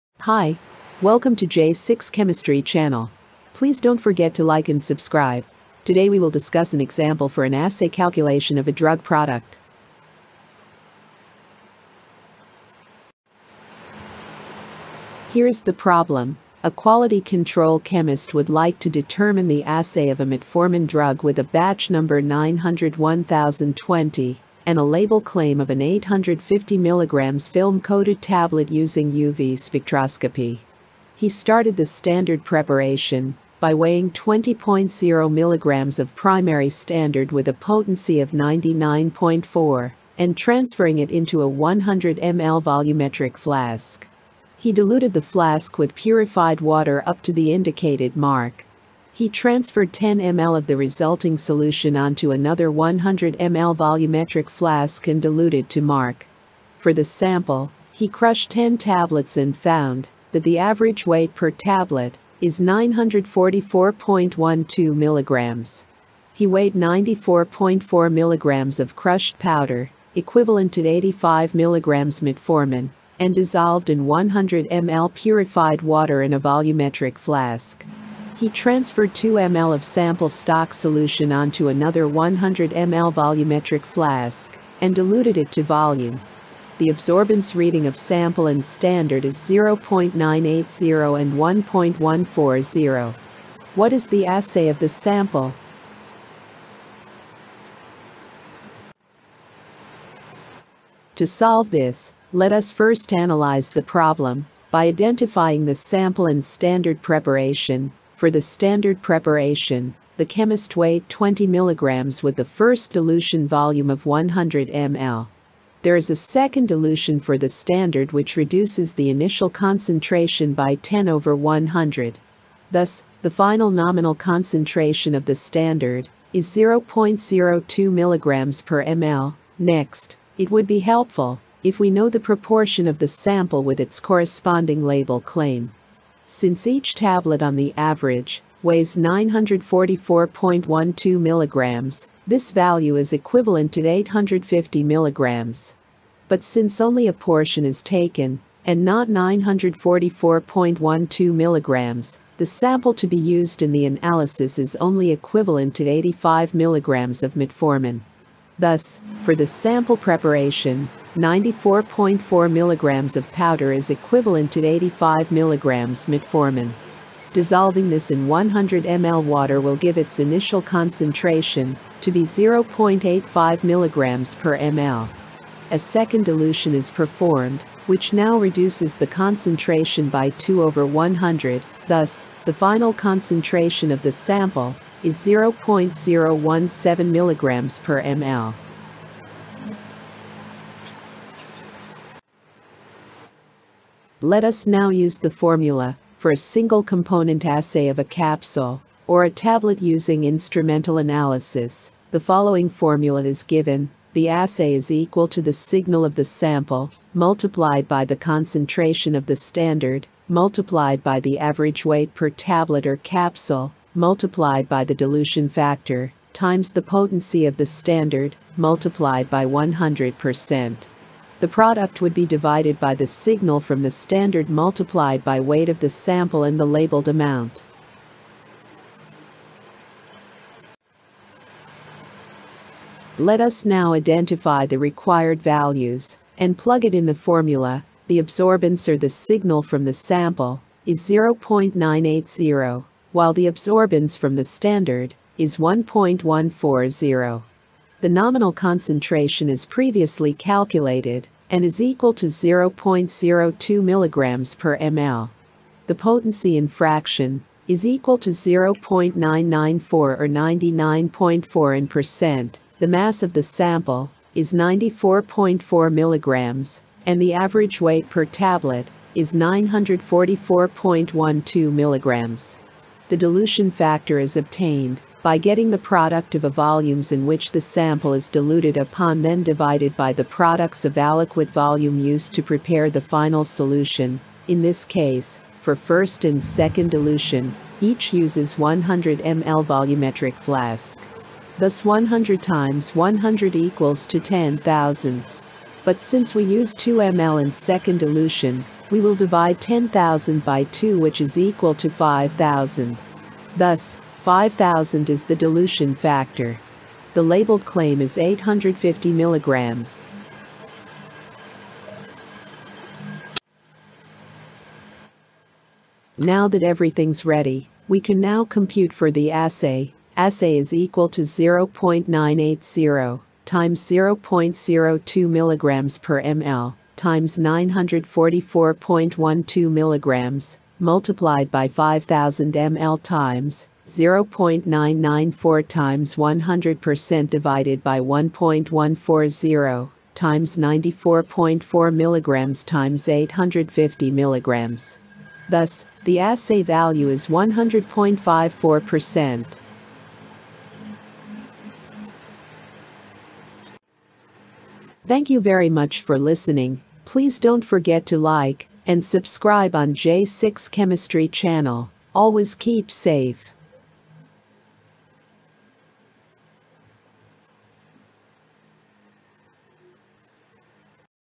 ASSAY CALCULATION BY UV SPECTROSCOPY (TEST COMPUTATION)
This is a sample problem with detailed solutions on "assay calculation" (computation) on drug product purity (Metformin Hydrochloride Tablet) by UV-Visible Spectroscopy.